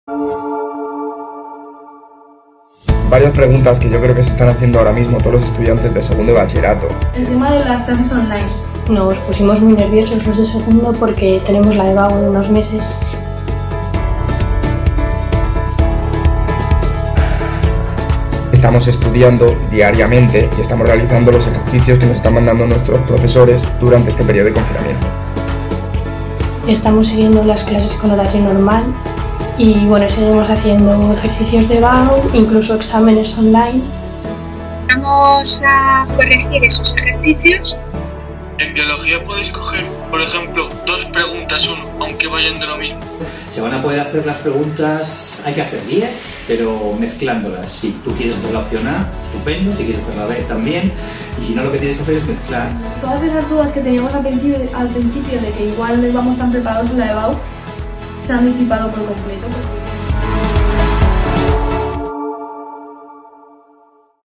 School Diary 17: Cómo preparar la EVaU 2020 [ EDICIÓN CORONAVIRUS ]
Nuestros alumnos de 2º de Bachillerato preparan la EvAU durante todo el año académico y no se han parado durante la cuarentena. Se han adaptado a las clases online y sus profes les siguen guiando ante este escenario diferente. ¡Descubre sus consejos!